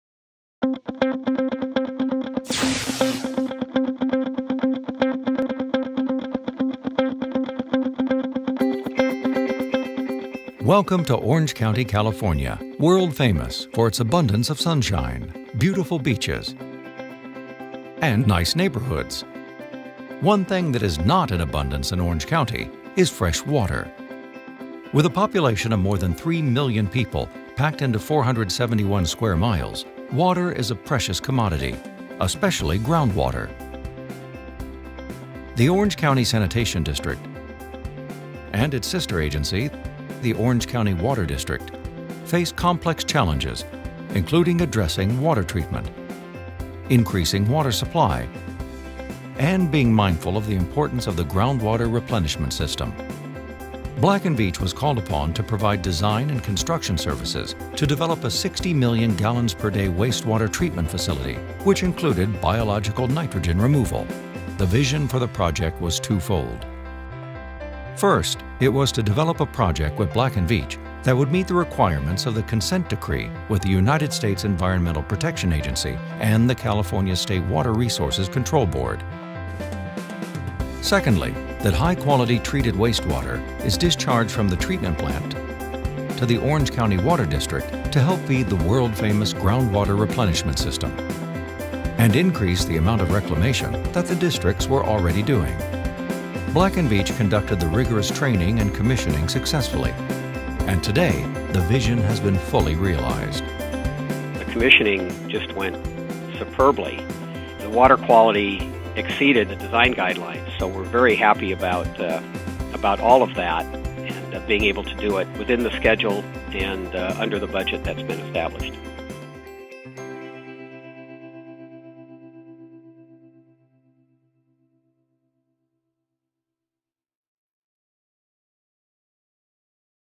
Orange County Sanitation District - Replenishing Precious Groundwater
Black & Veatch was called upon to help the OCSD with design and construction services to develop a 60-million-gallons-per-day wastewater treatment facility.

[Transcript]
Announcer: Welcome to Orange County, California. World famous for its abundance of sunshine, beautiful beaches, and nice neighborhoods.

One thing that is not in abundance in Orange County is fresh water.

With a population of more than 3 million people packed into 471 square miles, water is a precious commodity ... especially groundwater.

The Orange County Sanitation District ...and its sister agency --  the Orange County Water District faced complex challenges including addressing water treatment,
increasing water supply, and being mindful of the importance of the Groundwater Replenishment System.

Black & Veatch was called upon to provide design and construction services to develop a 60-million-gallons-per-day wastewater treatment facility, which included biological nitrogen removal.

The vision for the project was twofold.

First, it was to develop a project with Black & Veatch that would meet the requirements of the consent decree with the United States Environmental Protection Agency and the California State Water Resources Control Board.

Secondly, that high-quality treated wastewater is discharged from the treatment plant to the Orange County Water District to help feed the world-famous Groundwater Replenishment System.

And increase the amount of reclamation that the Districts were already doing.

Black & Veatch conducted the rigorous training and commissioning successfully. And today, the vision has been fully realized.

Jim Clark, Black & Veatch: The commissioning just went superbly, and the water quality exceeded the design guidelines, so we're very happy about all of that and being able to do it within the schedule and under the budget that's been established."